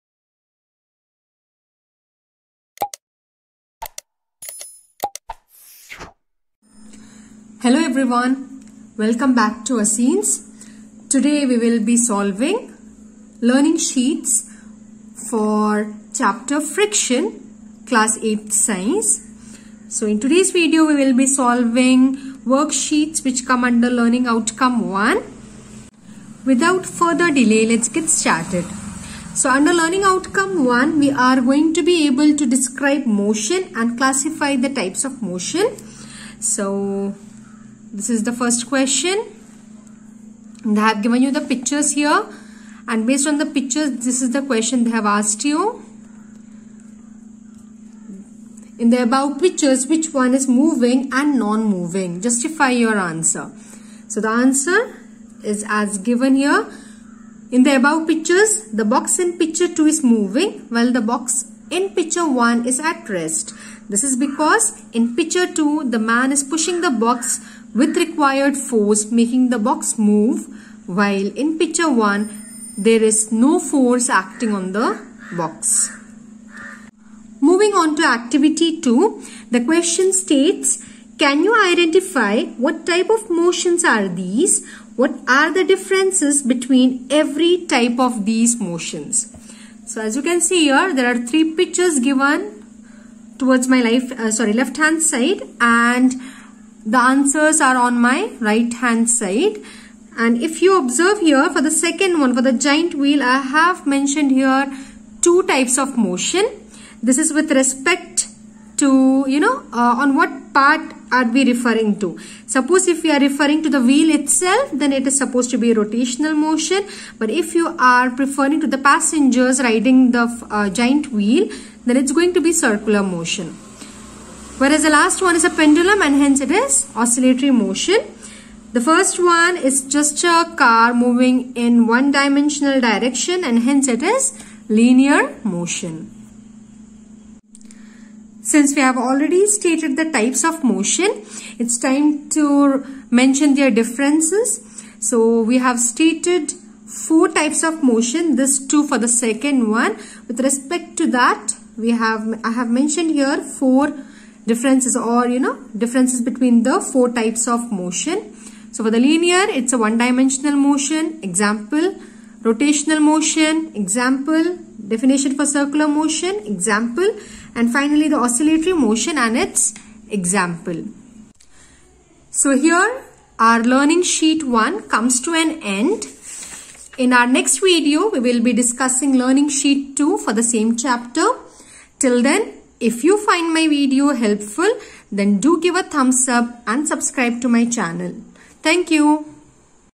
KALIKA CHETHARIKE || CLASS 8 SCIENCE || FRICTION || LEARNING SHEET 1
This video contains solved worksheets of learning outcome 1 class 8 science Friction chapter.

Further worksheets will be solved in upcoming videos.